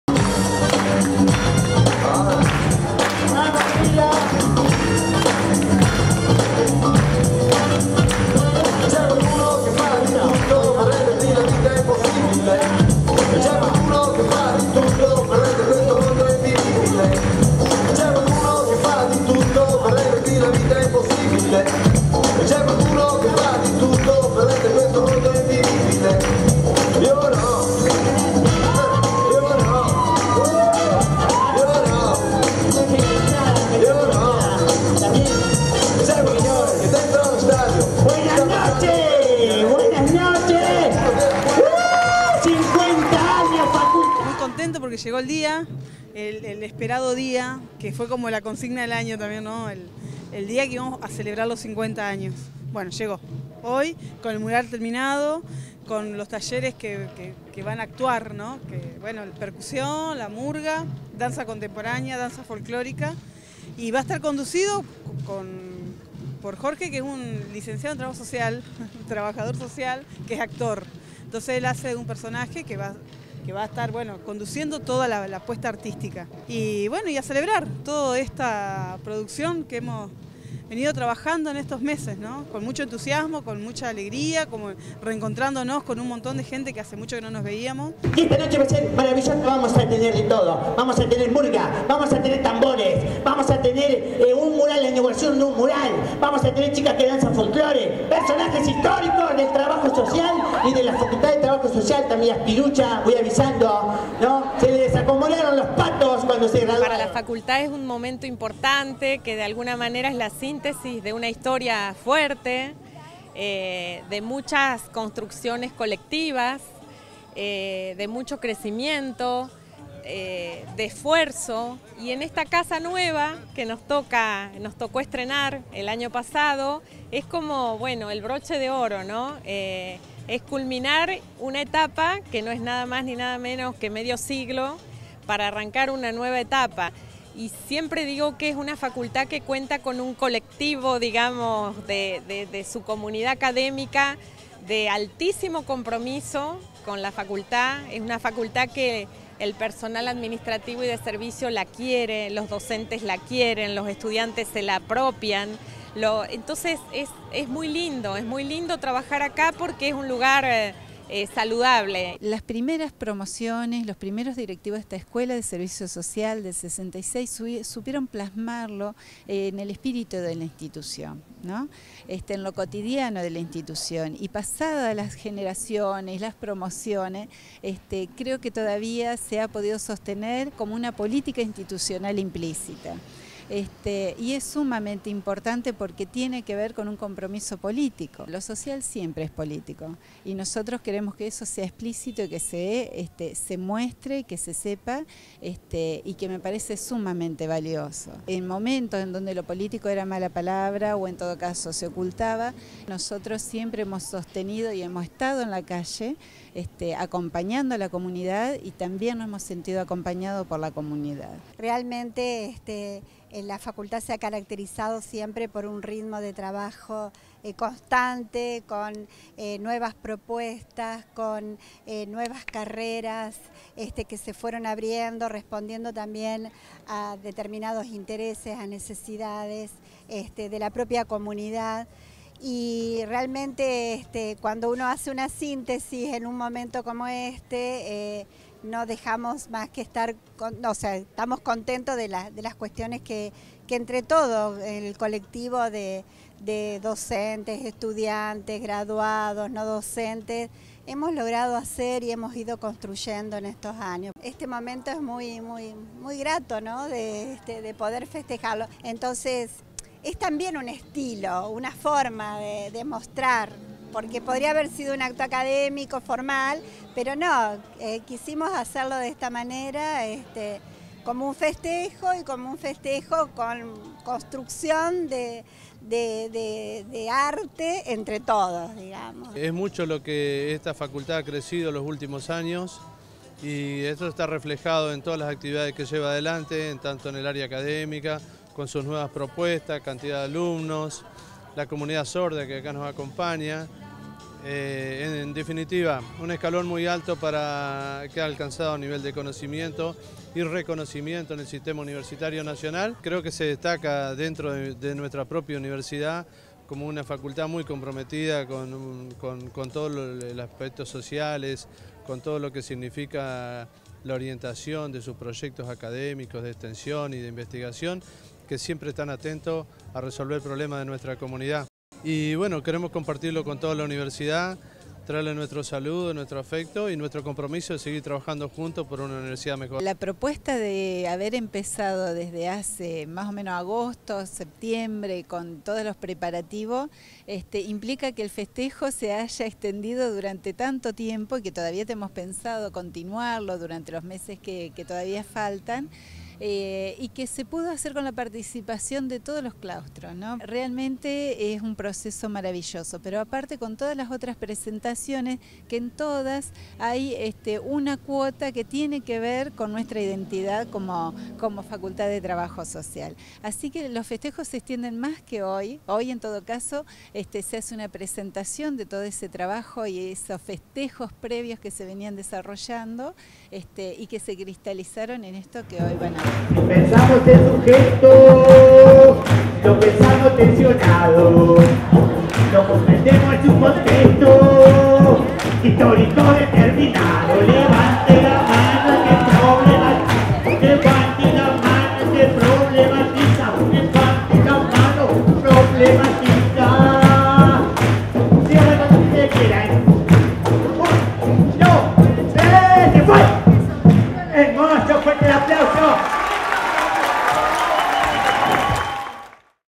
La Facultad de Trabajo Social celebró sus 50 años
La Facultad de Trabajo Social de nuestra Universidad celebró sus 50 años de vida institucional con un acto central en su nueva sede. La celebración fue compartida por toda la comunidad académica y de la ciudad, con diferentes expresiones artísticas.

El encuentro fue con una fiesta kermés que incluyó producciones audiovisuales, danza contemporánea y áerea, un cuento bilingüe (Lengua de Señas Argentina), danza folclórica, percusión, murga y la inauguración de un mural conmemorativo. La jornada finalizó con un baile.

Cabe señalar que durante tres meses el área Cultura, dependiente de la Secretaría de Extensión e Investigación de la facultad, desarrolló talleres de percusión, danza folklórica y mularismo con importante participación de actores institucionales, para la organización del festejo.